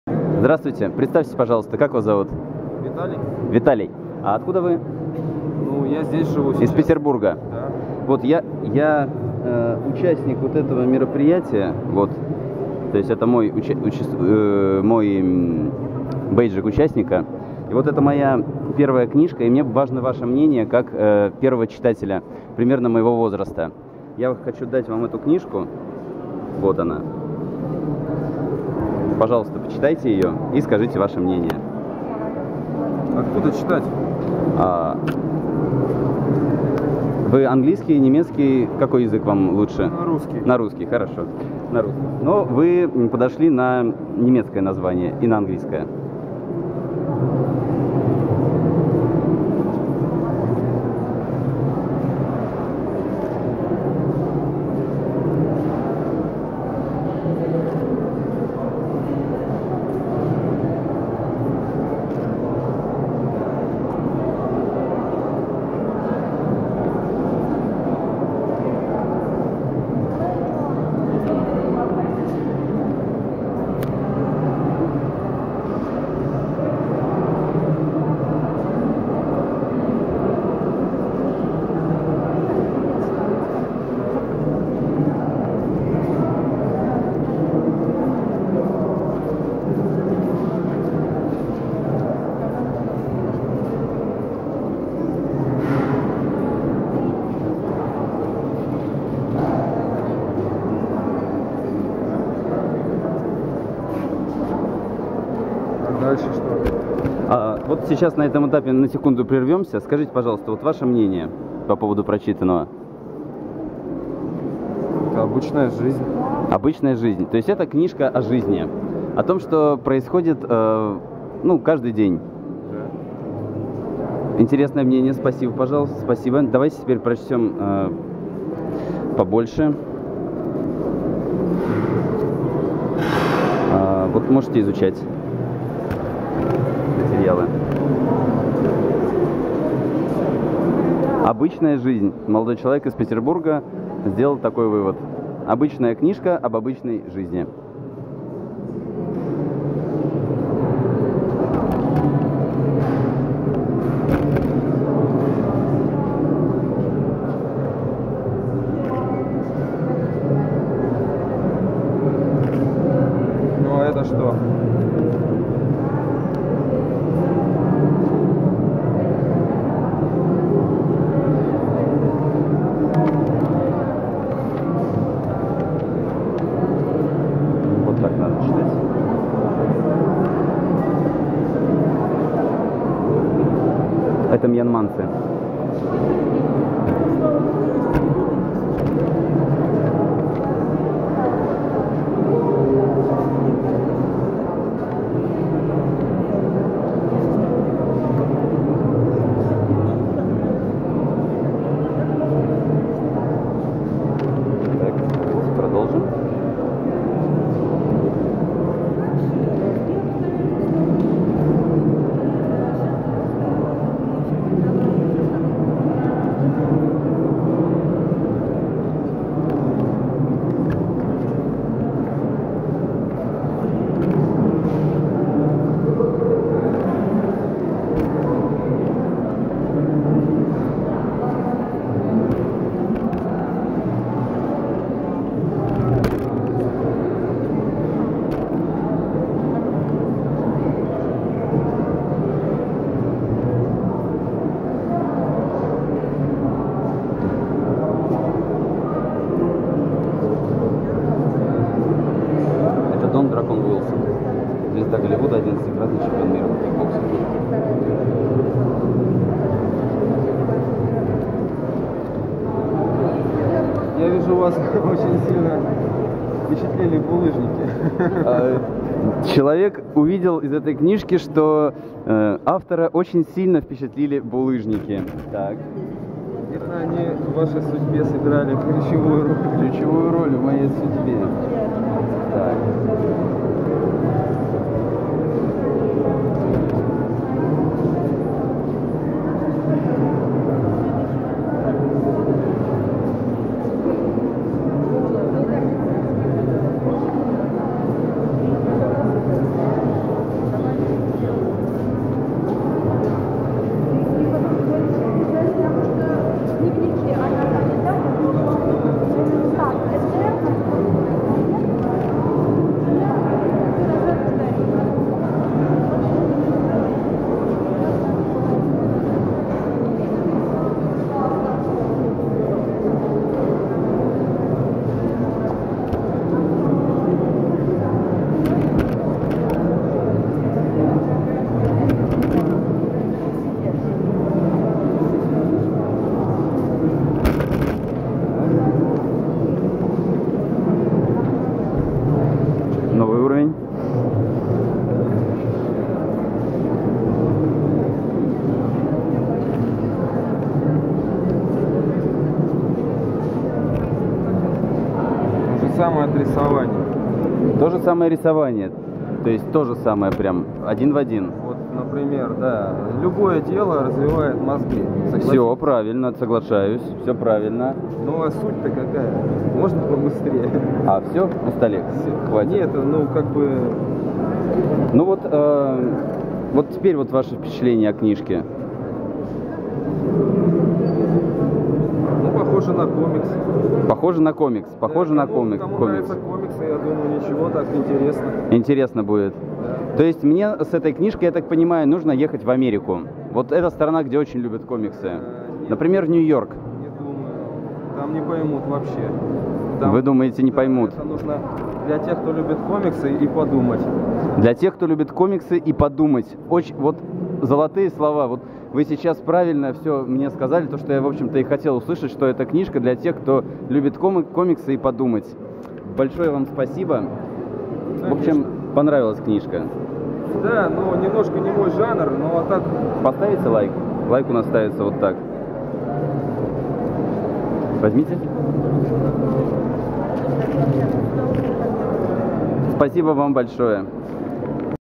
Мнение читателя о книге УРОКИ ЖОНГЛИРОВАНИЯ БУЛЫЖНИКАМИ
По мнению посетителя 9-го Санкт-Петербургского международного книжного салона
УРОКИ ЖОНГЛИРОВАНИЯ БУЛЫЖНИКАМИ
"...Это книга для тех кто любит комиксы и подумать. "

Спешите прочитать и приобрести!